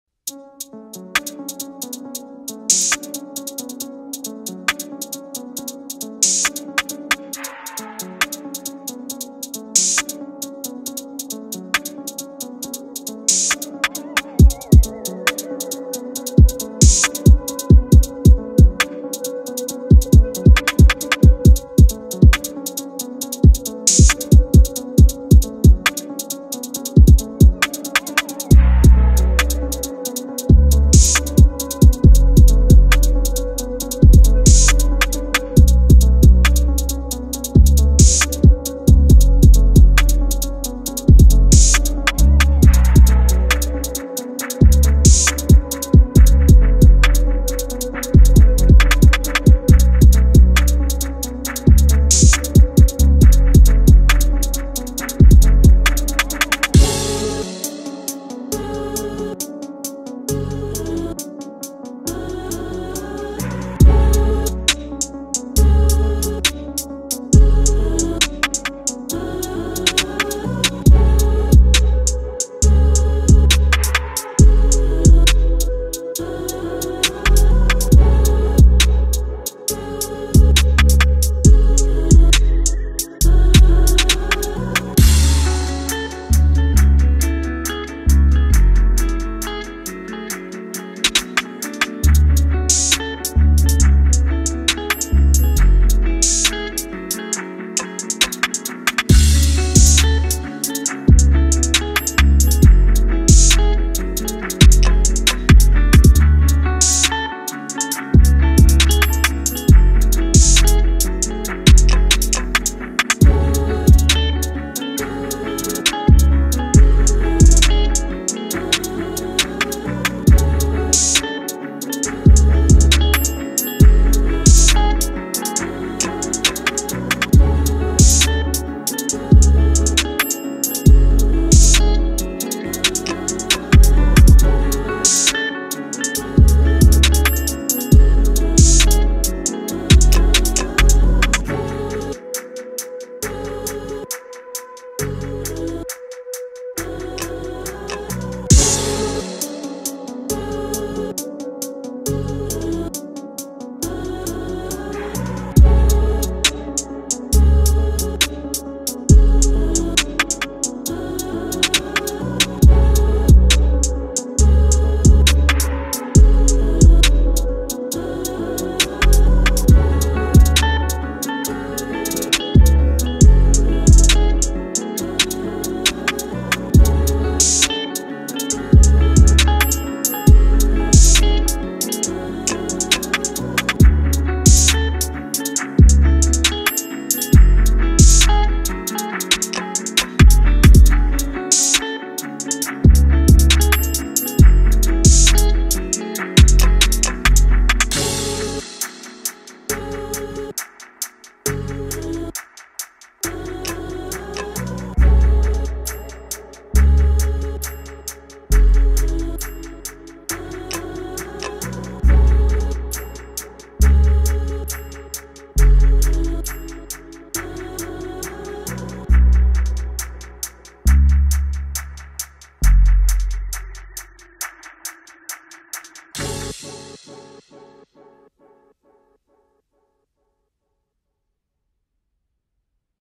Drill beat instrumental. Samples by @MarcusElbow and my big homie @THEBEATLEAGUE Piano/guitar
Drill beat instrumental. Samples by @MarcusElbow ​⁠ and my big homie @emsthebeatleague7450 ​⁠ Drill-ish Piano/guitar type beat for artists to the likes of @CentralCee @MdotROfficialVEVO. See  PopsProductions on @Beatstars to download any music which fits your tits.